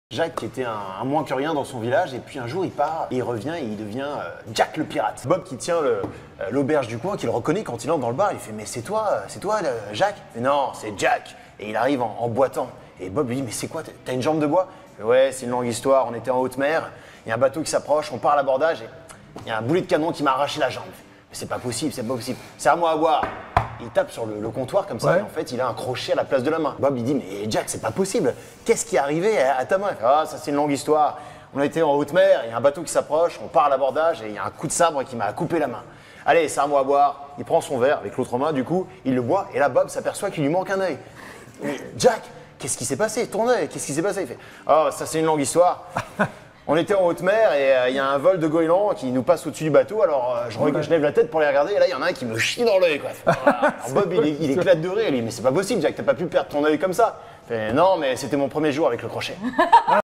Jack Le Pirate
Une de mes blagues préférées !

Merci à l’EXCELLENTE Marco Moustache d’avoir trouvé ça drôle !

Ceci dit : je n’en suis pas l’auteur, si vous voulez entendre les miennes (qui n’ont pas grand chose à voir) direction World Man Bio